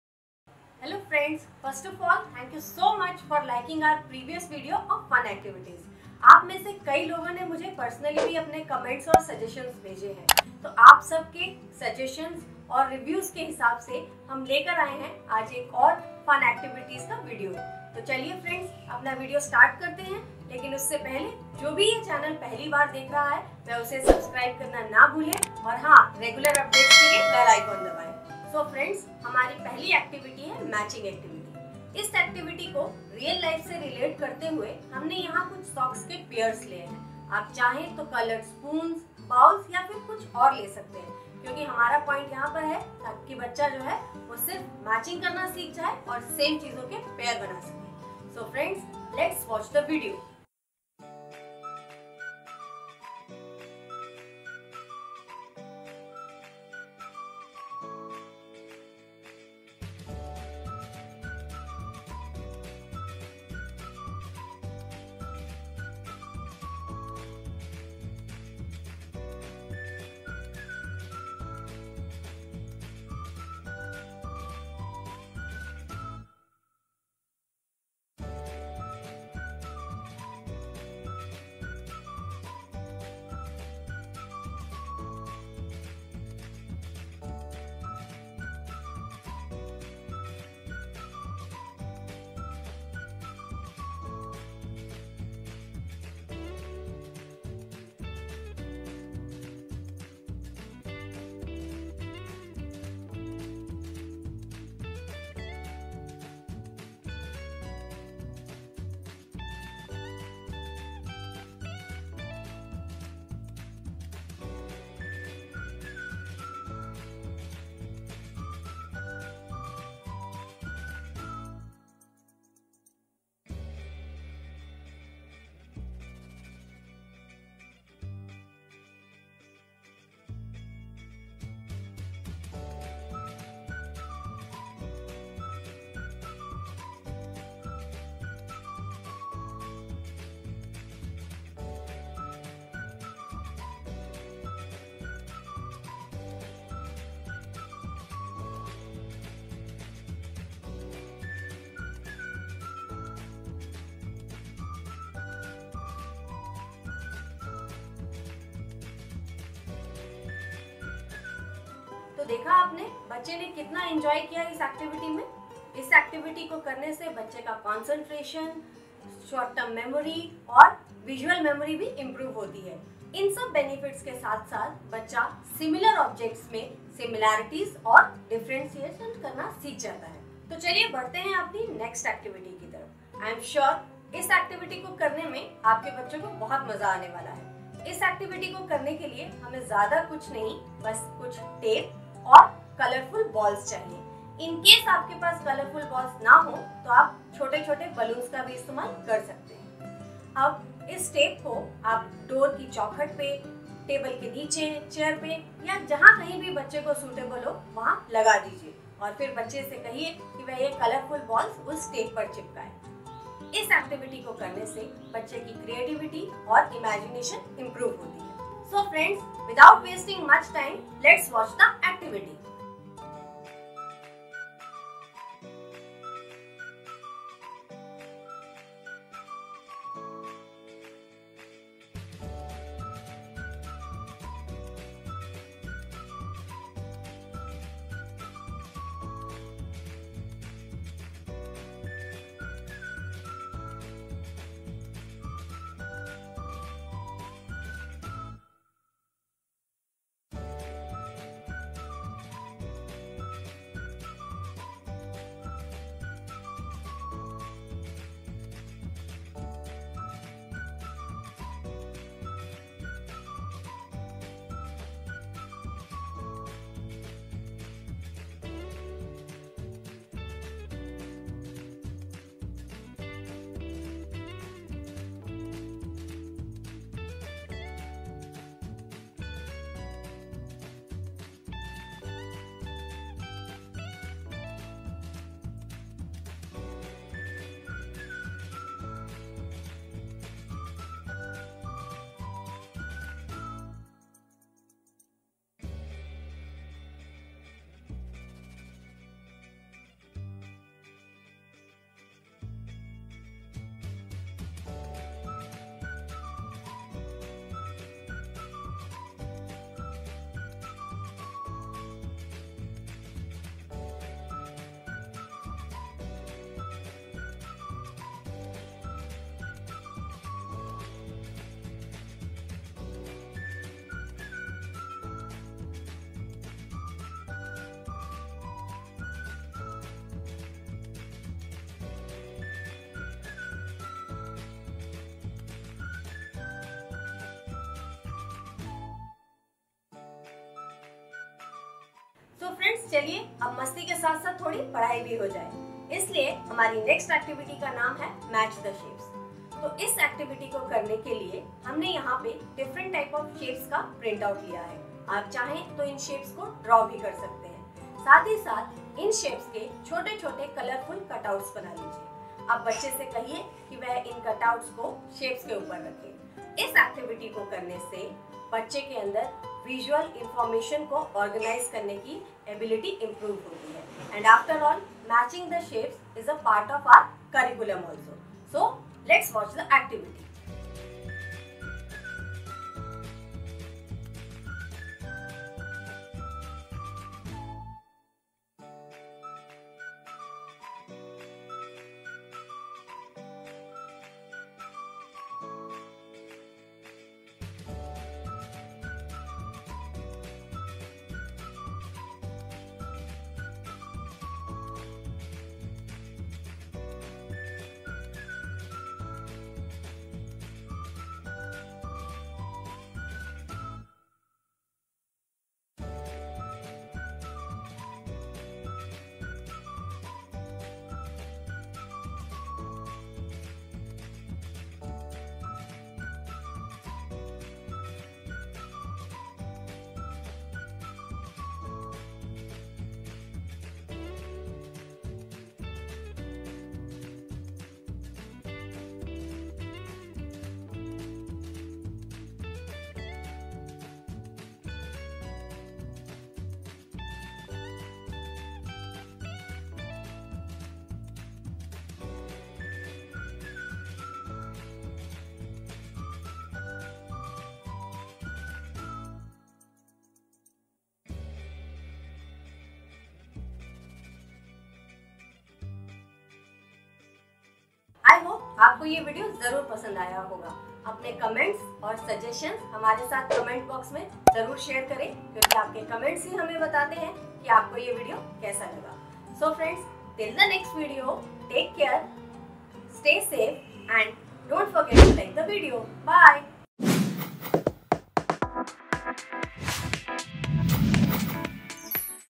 Fun Activities (Part - 2)
Every child likes the fun. They want to enjoy each and every moment and parents do their best to help their children to achieve every happiness.

This video also shows few activities which will not only give enjoyment to children also will improve their memory skills, visual memory and hand eye coordination.

The materials needed for today's activities

Colourful Balls
Tape
Cotton Ball
Socks (various pairs)